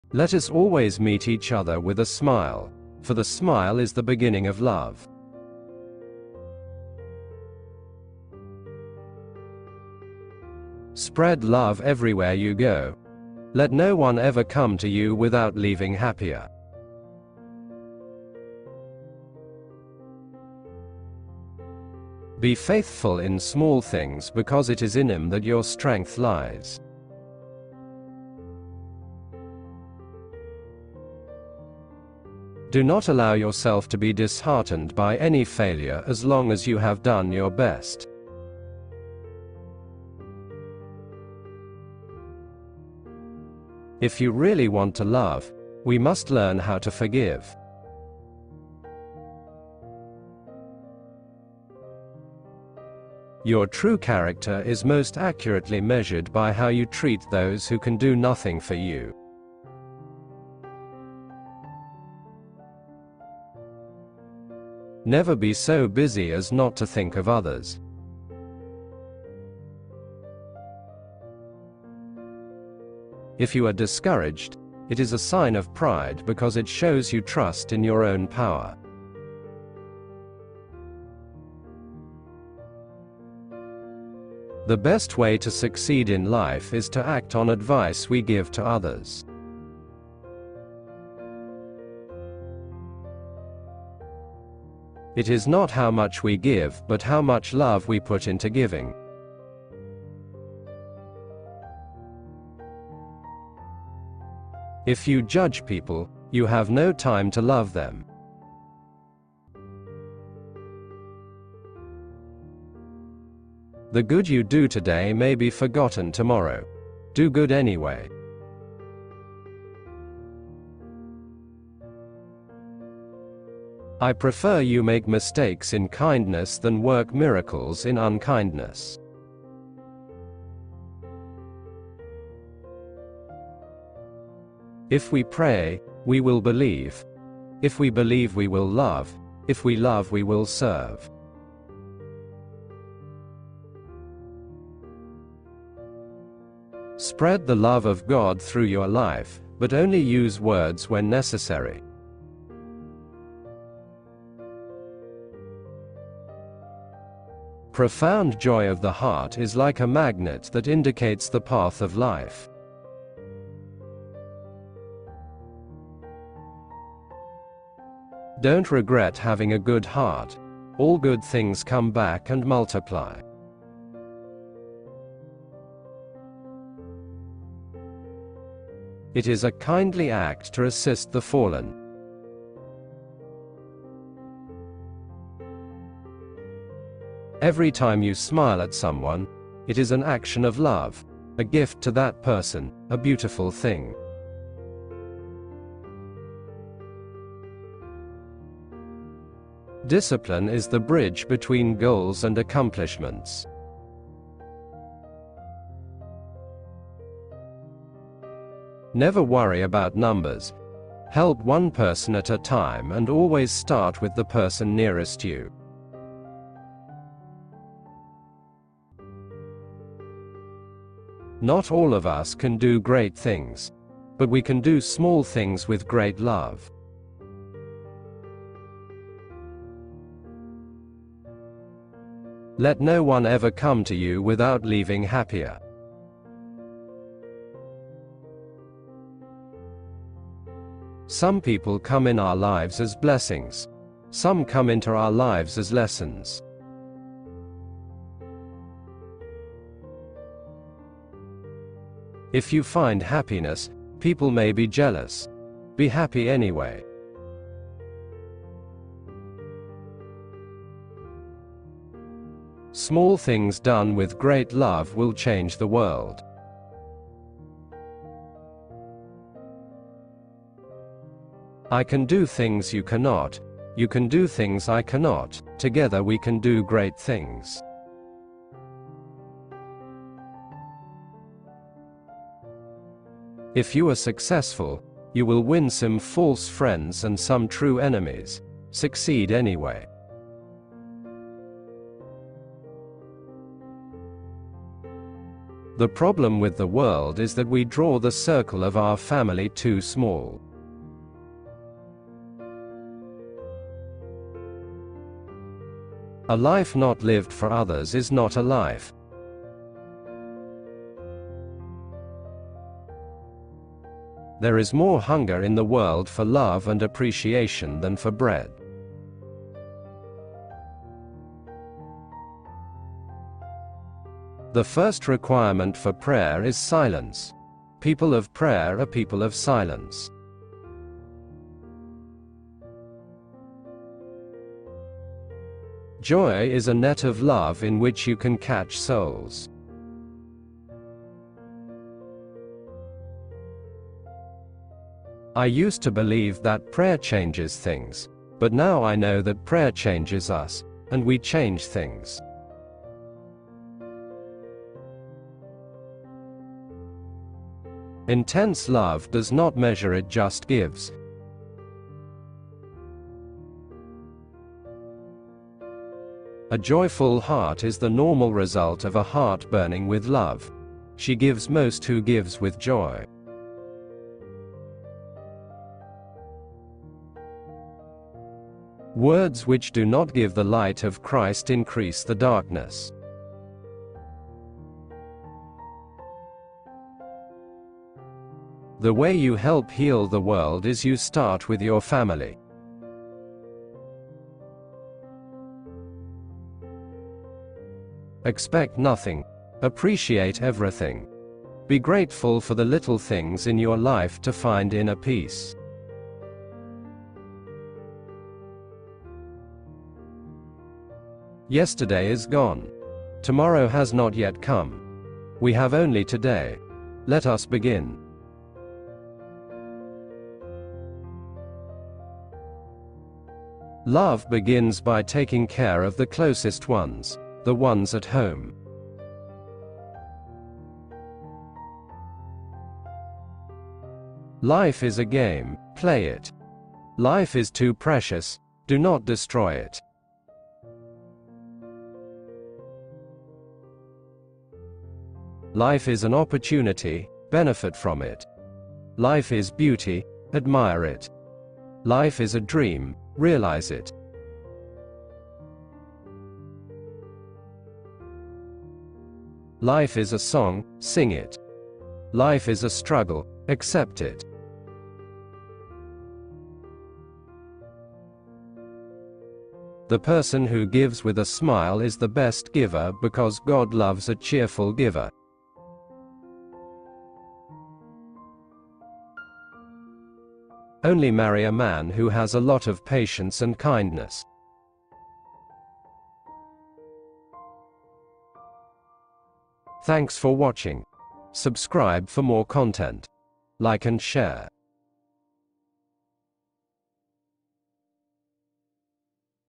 MOTHER THERESA'S QUOTES THAT ARE INSPIRING AND HELPFUL
Through her simple yet profound quotes, Mother Teresa teaches us that love, compassion, and service to others are the essence of a fulfilling life. Her words remind us that while we may not be able to change the entire world on our own, we can certainly make a difference in our small ways, and this ripple can eventually lead to a wave of change.